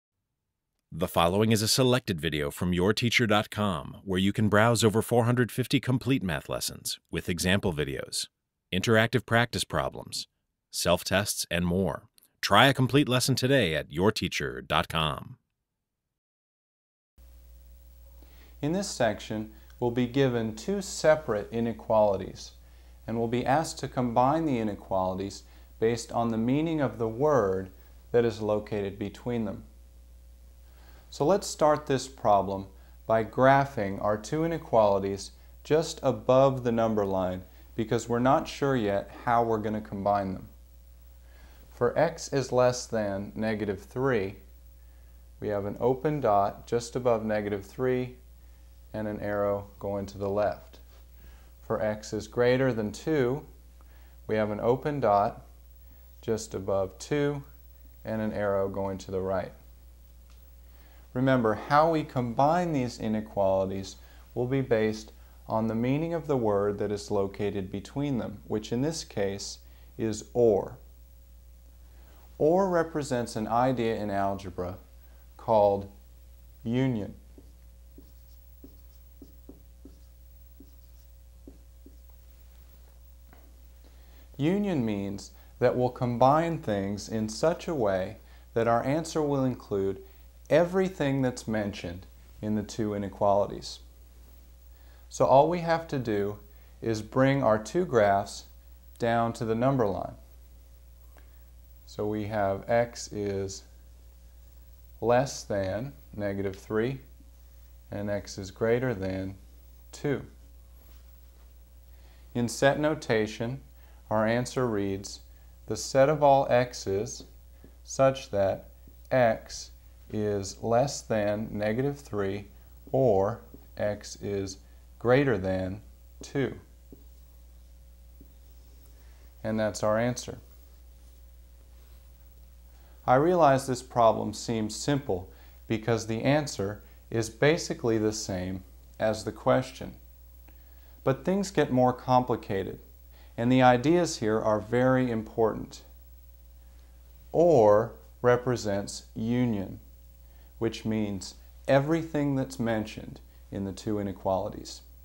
Algebra Refresher - MathHelp.com - 1000+ Online Math Lessons
MathHelp.com - is a great Algebra refresher with 1000+ online math lessons featuring a personal math teacher inside every lesson!